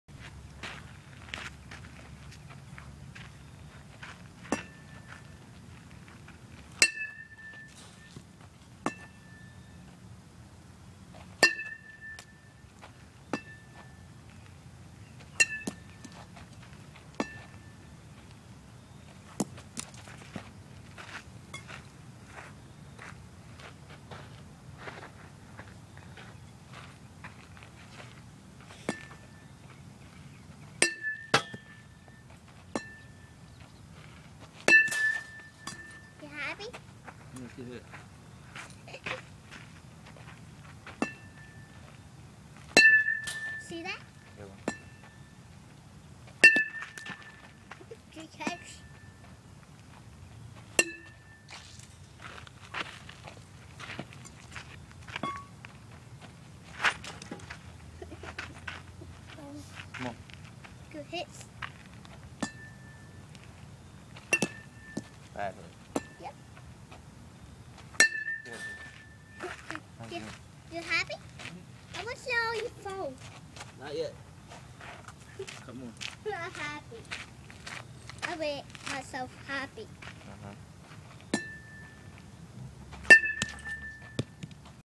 4 year old crushing baseballs
Young Gil in batting cages taking cuts.